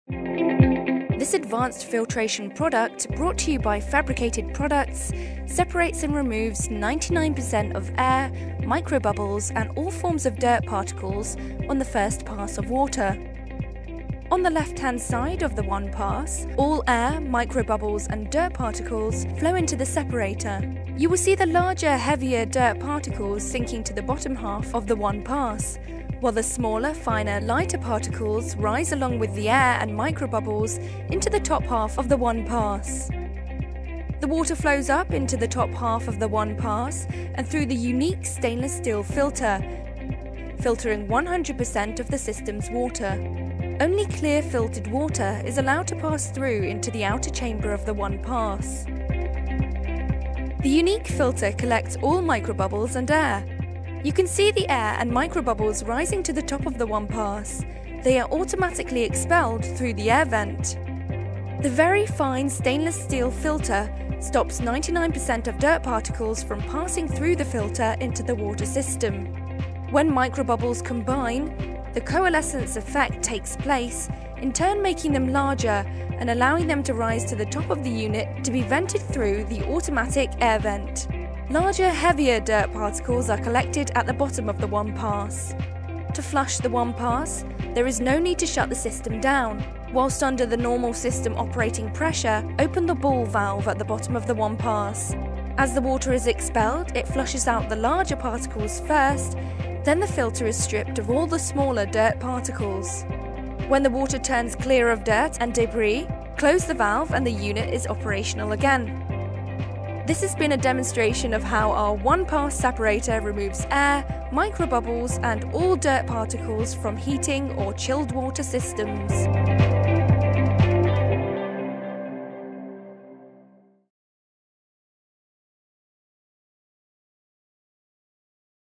How the 1Pass works
A demonstration of how the 1PASS air and dirt separator works.